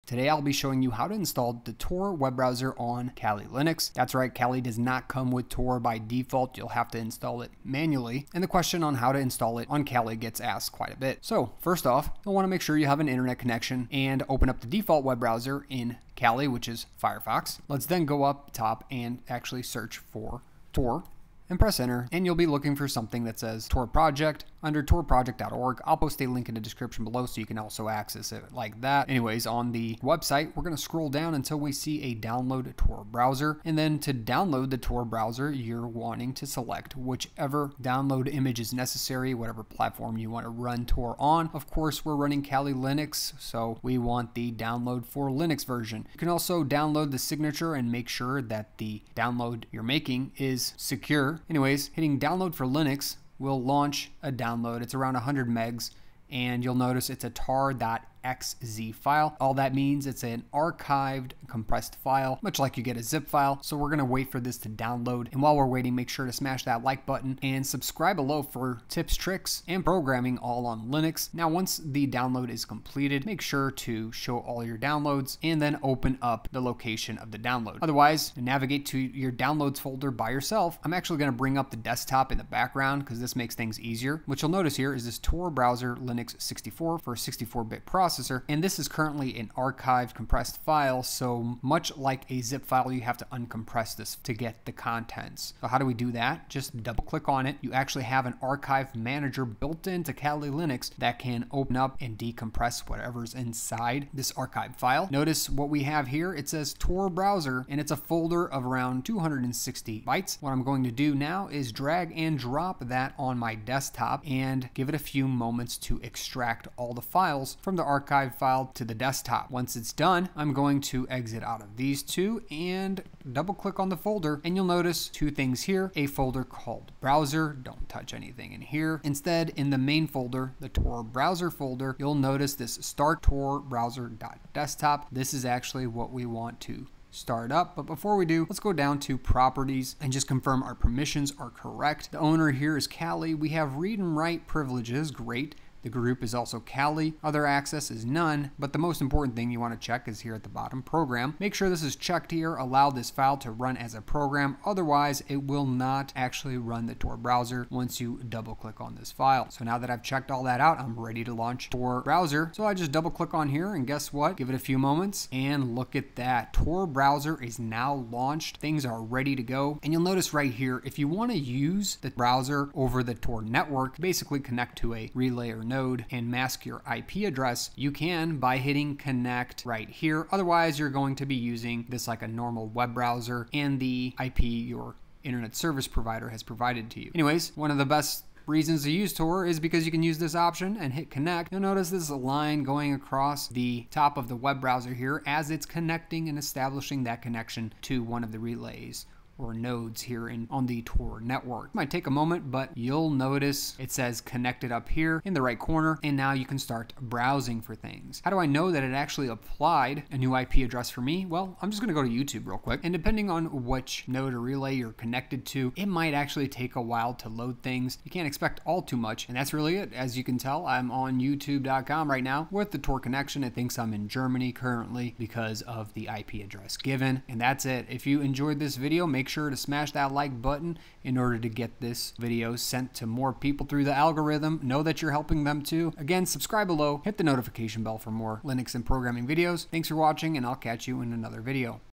How to Install TOR Browser on Kali Linux
Install and use the TOR web browser on Kali Linux. A step by step guide on downloading and installing TOR for your web browsing needs.